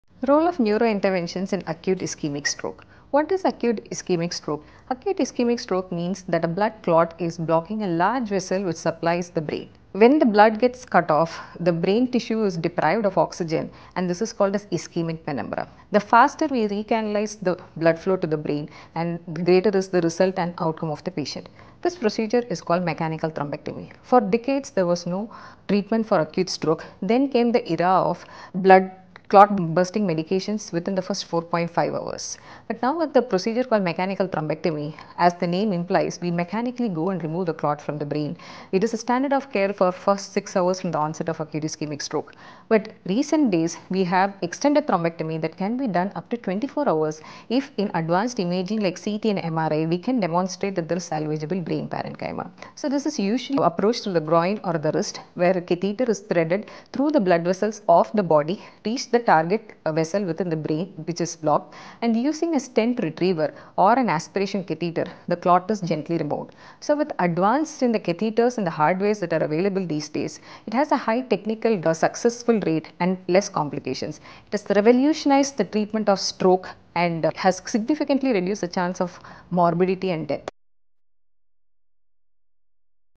Role of NeuroIntervention in Acute Ischemic Stroke|Every second counts when it comes to saving brain
In this episode, Dr. Aarthi Deepesh, Senior Consultant and Clinical Head – Neuroimaging & Interventional Radiology at MGM Healthcare, explains how timely neuro-interventional procedures can restore blood flow and transform outcomes for patients with acute ischemic stroke.

Precision. Speed. Expertise - that’s the power of Interventional Radiology in action.

To consult with Dr Aarthi Deepesh, call MGM Healthcare at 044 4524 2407